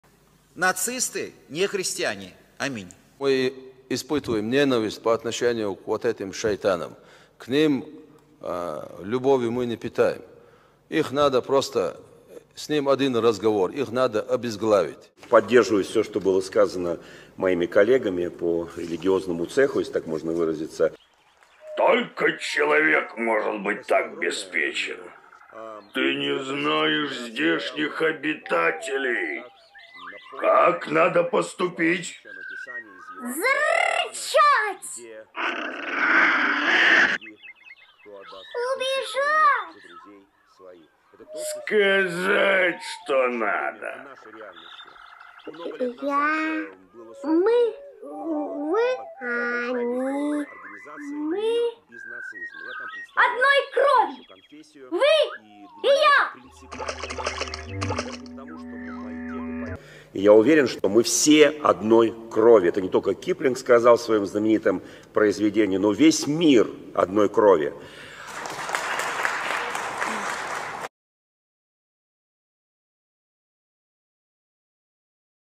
Что нужно власти, то и говорит Сергей Ряховский | Мы с ним не одной крови
Замечательный образ придумал для своего выступления на круглом столе в Государственной Думе России по завуалированной под осуждение нацизма оправдание агрессии против Украины и проведения СВО сам Сергей Ряховский. Как вести себя в стае Маугли учит медведь Балу. Как вести себя среди коллег по религиозному цеху знает начальствующий епископ РОСХВЕ Сергей Ряховский.
К сожалению, из всего хорошего, что он сделал за десятилетия прибывания на своем посту в памяти останется его последние шаги и выступления, которые послужили расколу некогда перспективного Союза.
Вы можете поддержать нас финансово:
YouTube канал ВЗГЛЯД С НЕБЕСНОЙ